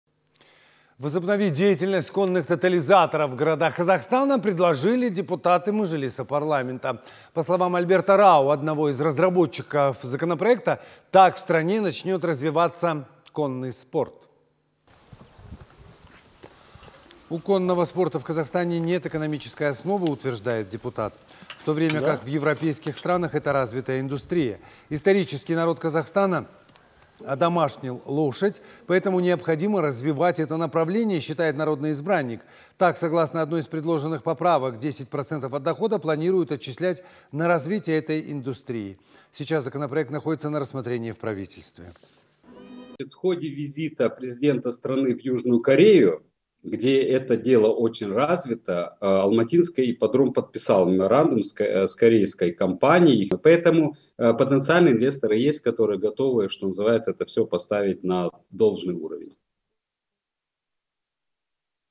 Депутаты предложили вернуть тотализаторы
Возобновить деятельность конных тотализаторов в городах Казахстана предложили депутаты мажилиса парламента. По словам Альберта Рау, одного из разработчиков законопроекта, так в стране начнет развиваться конный спорт.
У конного спорта в Казахстане нет экономической основы, утверждает депутат. В то время как в европейских странах это развитая индустрия. Исторически народ Казахстана одомашнил лошадь, поэтому необходимо развивать это направление, считает народный избранник. Так, согласно одной из предложенных поправок, 10% от дохода планируют отчислять на развитие этой индустрии. Сейчас законопроект находится на рассмотрении в правительстве.